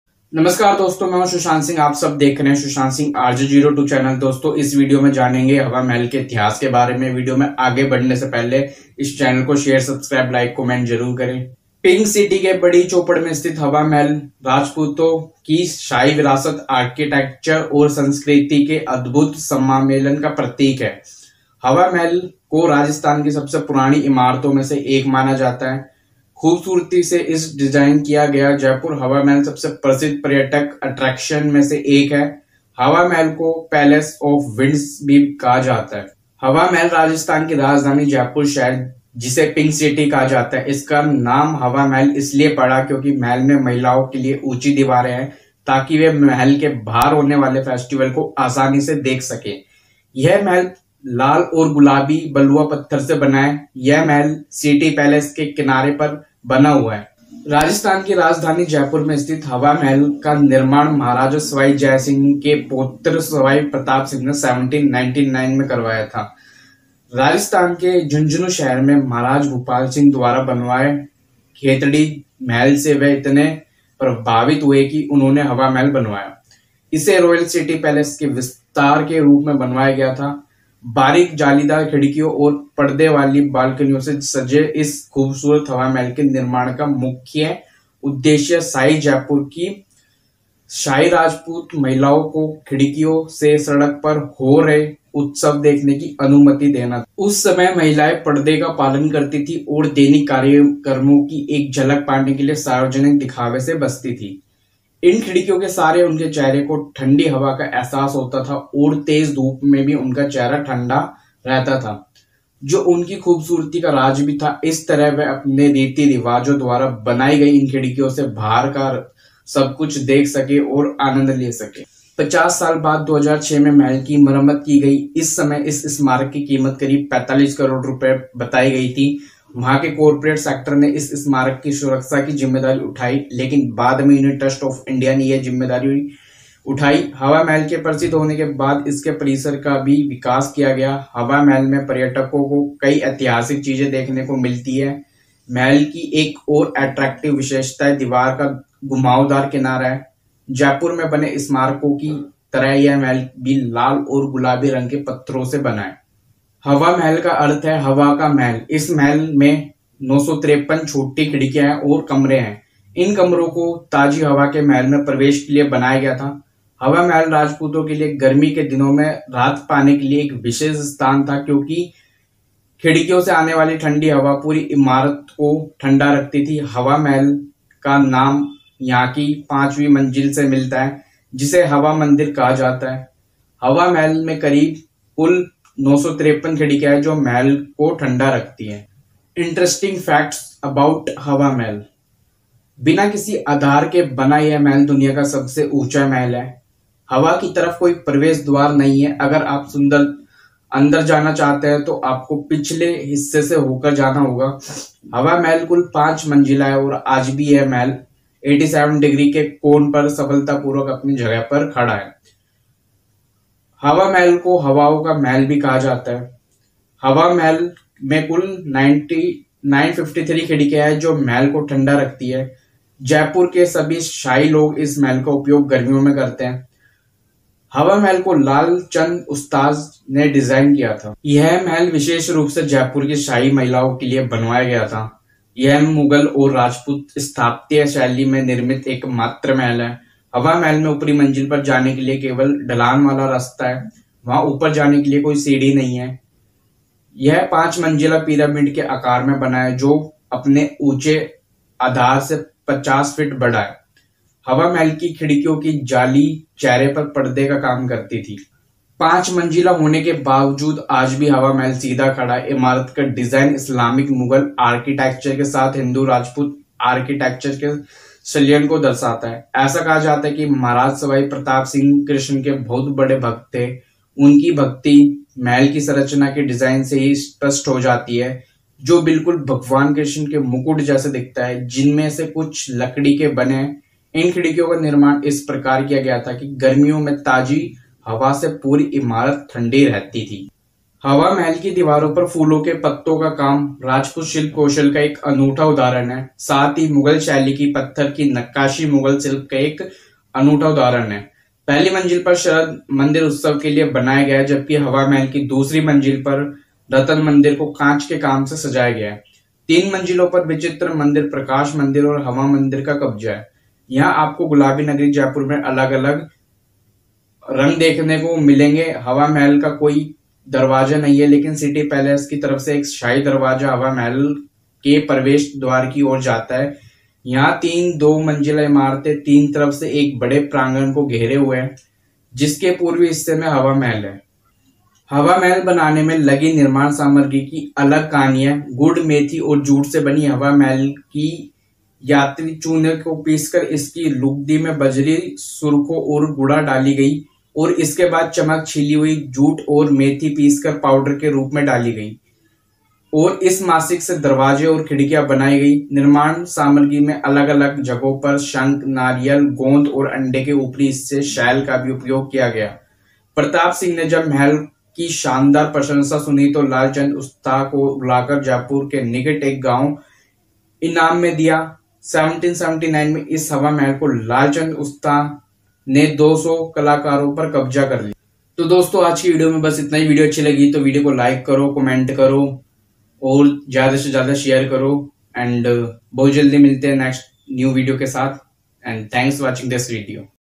||राजस्थान की राजधानी जयपुर में बना हवा महल|| राजपूतो के लिए गर्मी के दिनों में राहत|कुल 953 खिड़की|
Keep Supporting Love You All My Youtube Family😘